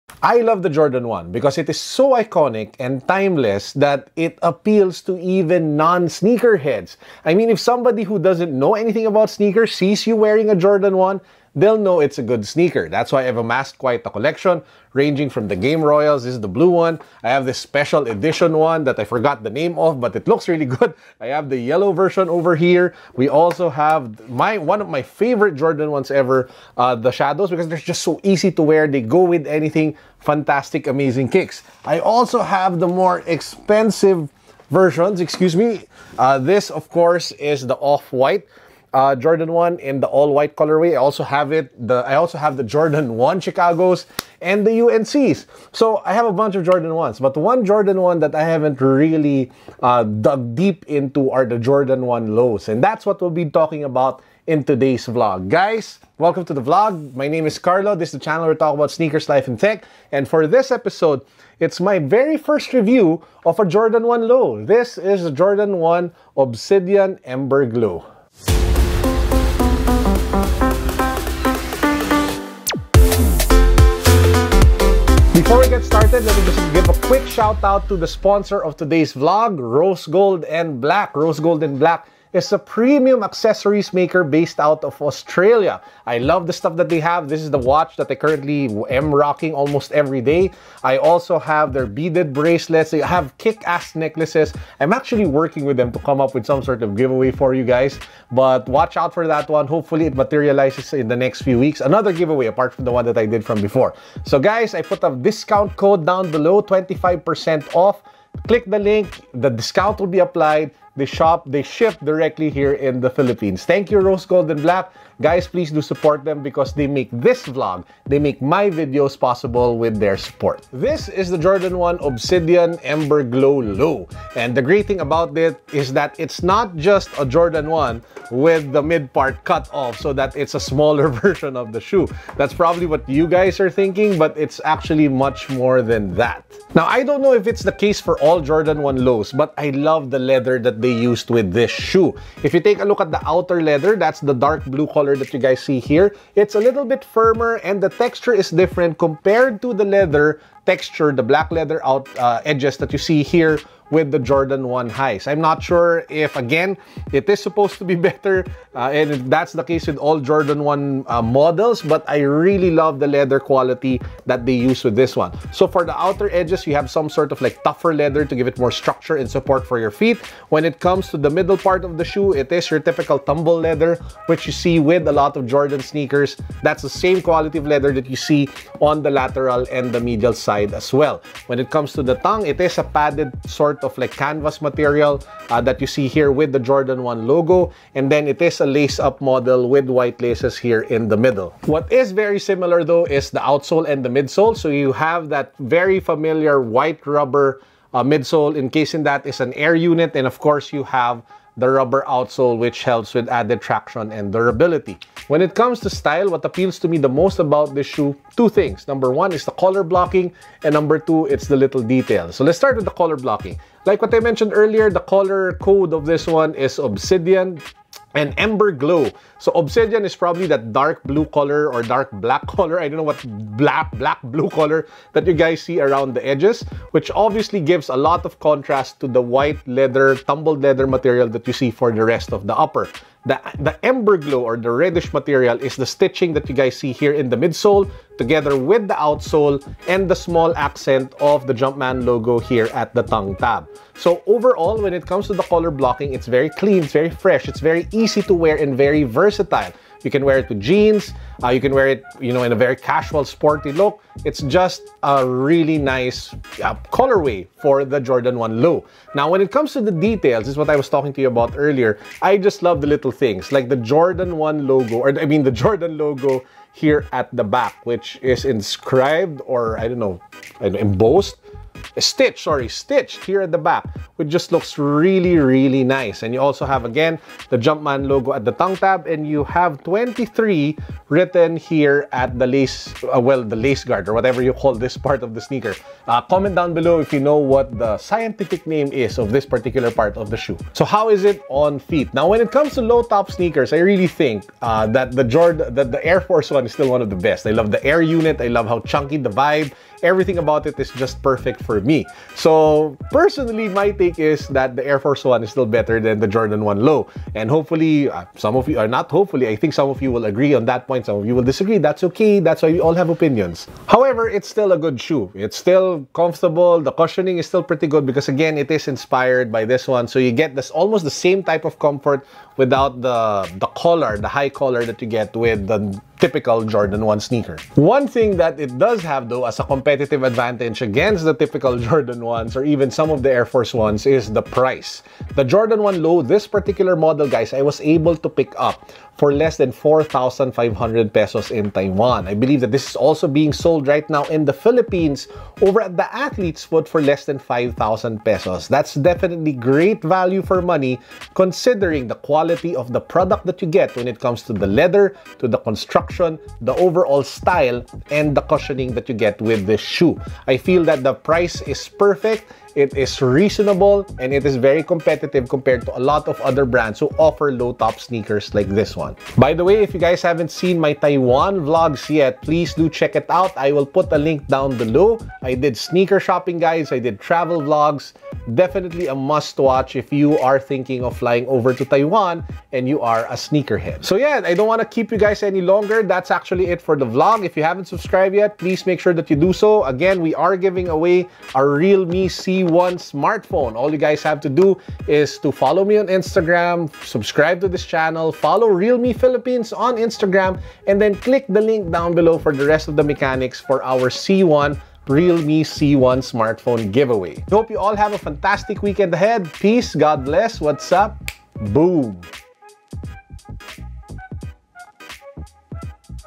SNEAKER STEAL DEAL FOR UNDER $80 (Php4,000): JORDAN 1 "EMBER GLOW" ON-FEET REVIEW
For this vlog we do an on-feet review of my best pick-up in Taiwan, the Jordan 1 Low "Ember Glow". I hope you guys enjoy this vlog!